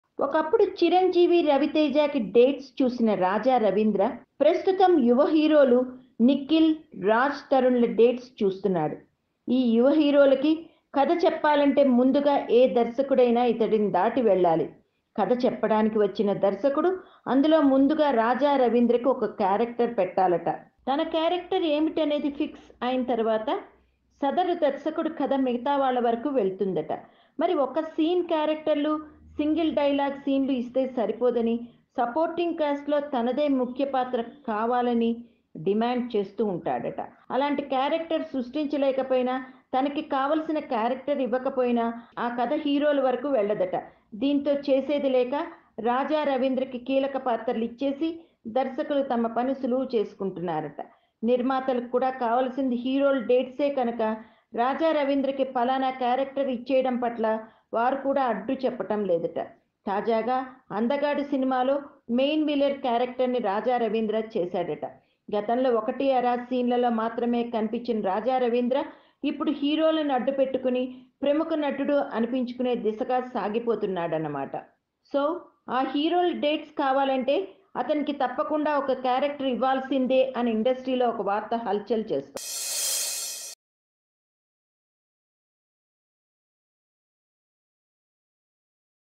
Raja Ravindra Blackmail on Directors | Tollywood Gossips | Tollywood Boxoffice TV
Raja Ravindra Shocking Demands From Tollywood Directors. Watch Video To Know What Did He Demands